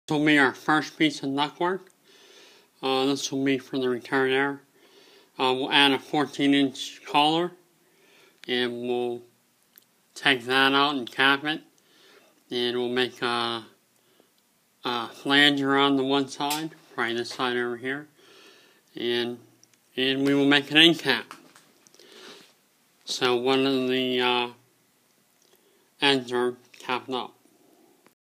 Using some old ductwork
Nothing like reusing ductwork instead of throwing it in the dumpster.